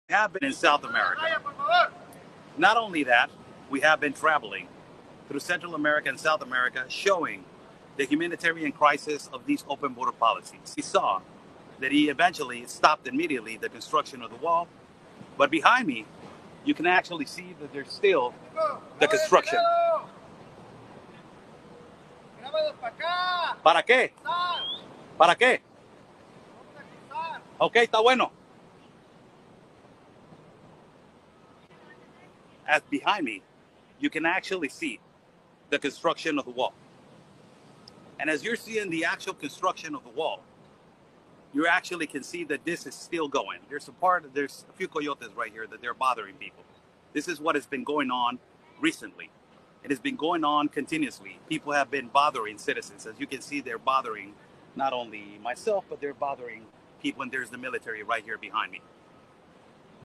"Coyotes" trying to disrupt
What you don't usually see behind the scenes. While @OscarBlueRamirez  reports near border walls, in caravans, in the Darien jungle, many times "Coyotes" are near, sometimes approach him, sometimes try to intimidate.